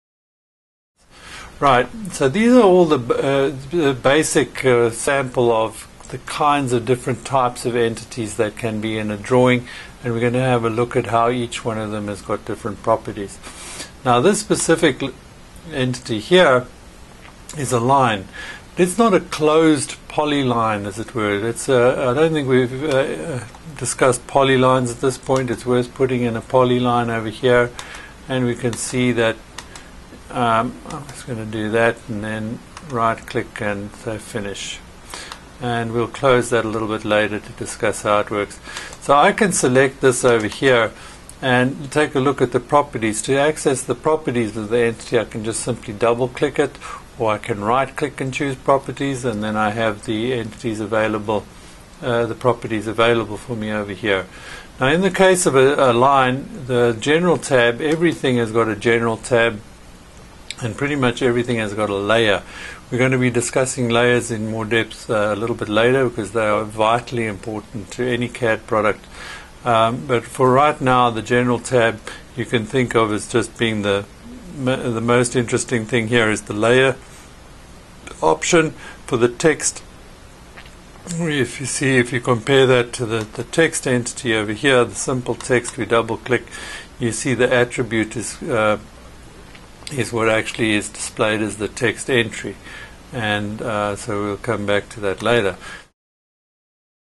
TurboCAD 19 Deluxe - 2D Core Concepts - Sample 01
The TurboCAD 19 Deluxe - 2D Core Concepts rGuide provides 2 hours and 34 minutes of detailed, instructor-led training on various topics, including the user interface, the advantages of using TurboCAD, keyboard shortcuts, item selection, copy commands, entities, the inspector bar, section tools, the edit tool, entity properties, modify tools, layers and more.